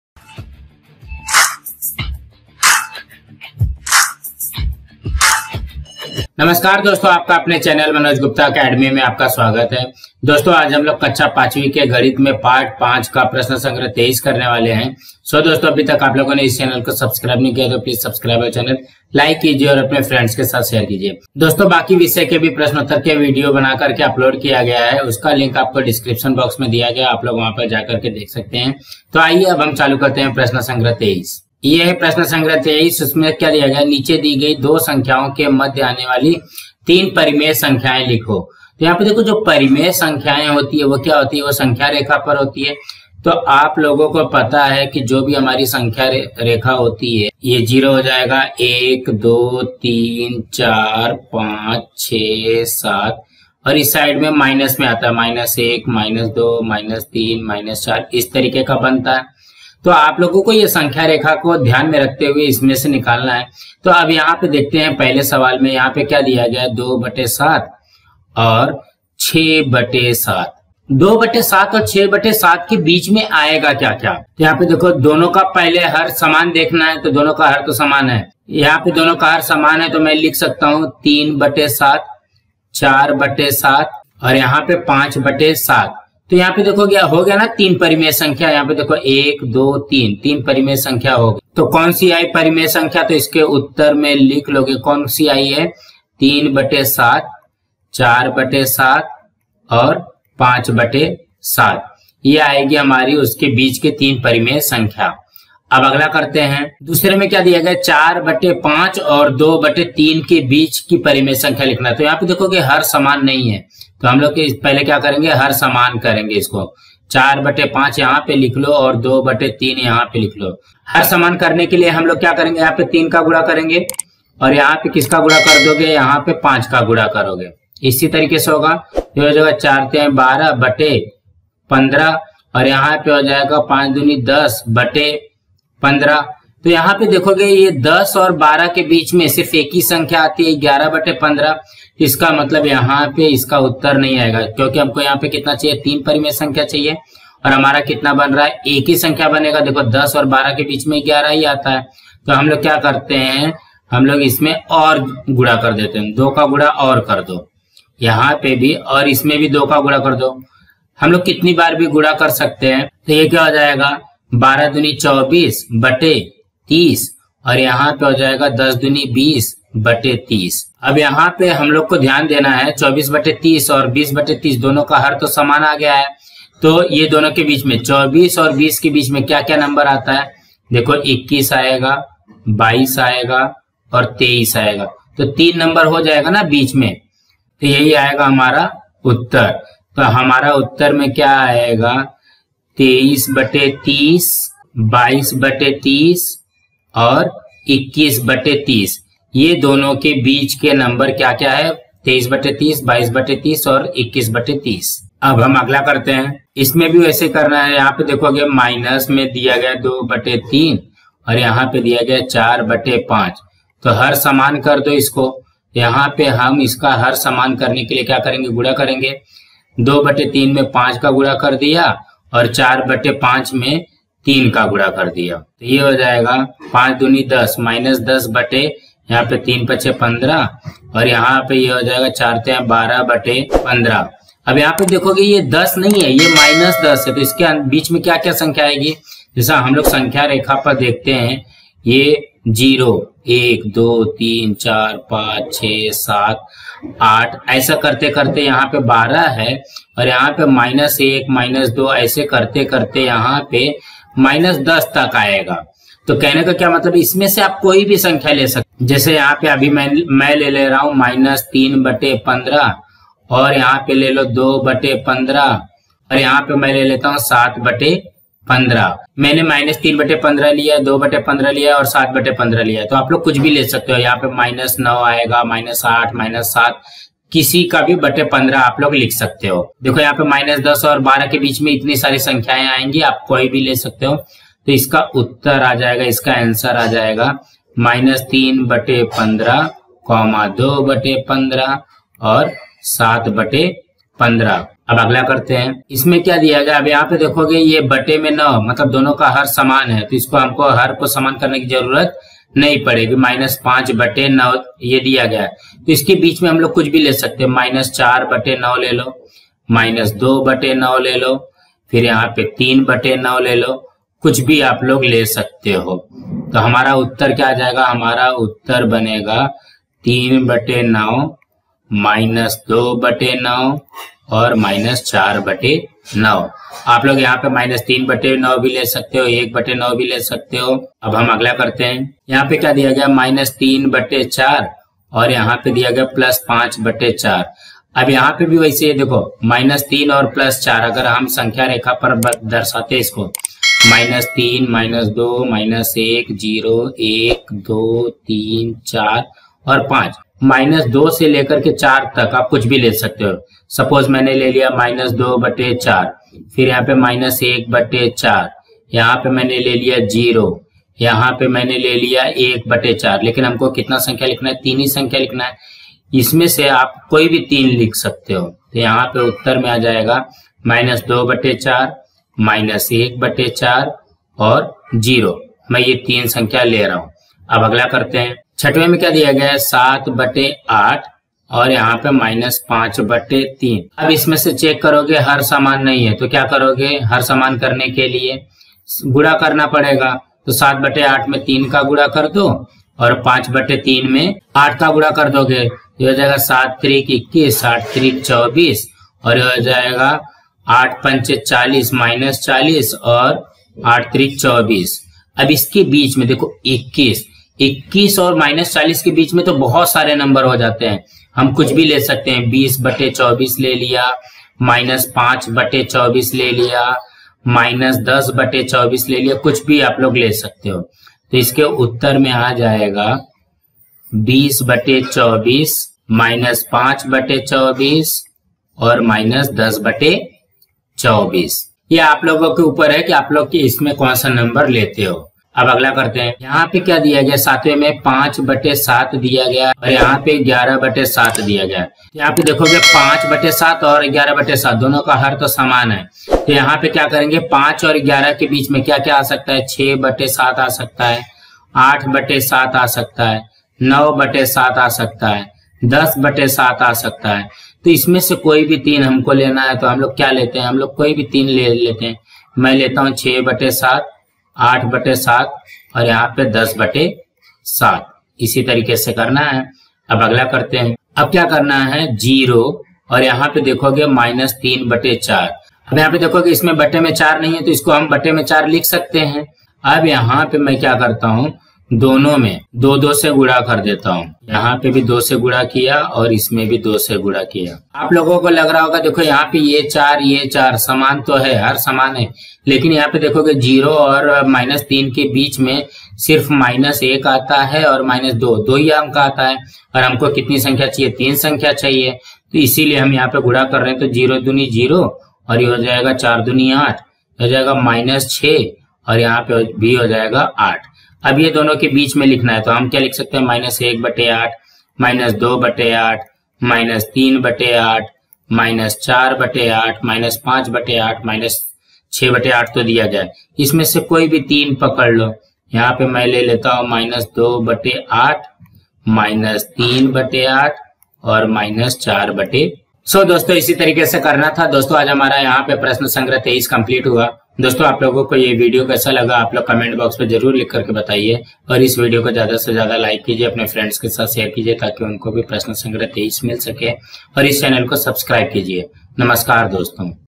प्रश्नसंग्रह- 23 गणित कक्षा- 7th परिमेय संख्याएँ और उन पर आधारित संक्रियाएँ Hindi Medium MH Board
Hello Friends

प्रश्नसंग्रह- 23 गणित कक्षा- 7th परिमेय संख्याएँ और उन पर आधारित संक्रियाएँ  Hindi Medium  Maharashtra Board